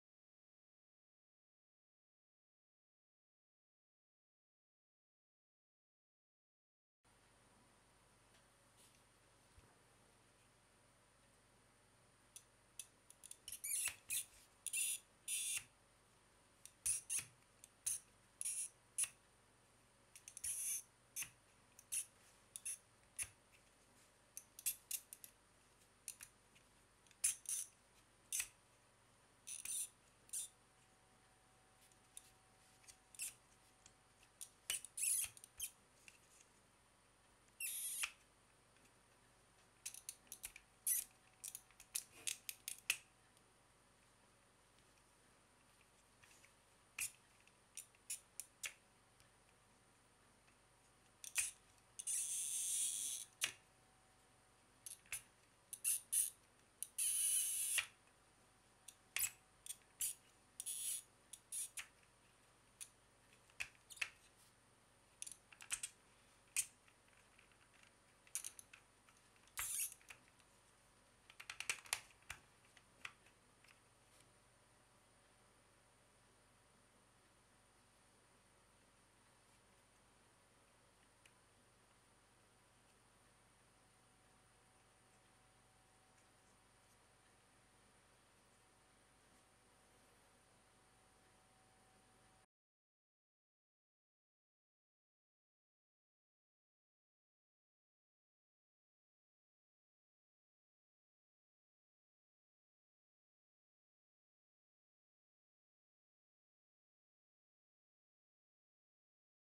Змейка Рубика Золотая рыбка | Rubik`s Snake Gold fish
Как сделать из Змейки Рубика золотую рыбку?
How to make a Rubik's Snake Gold fish?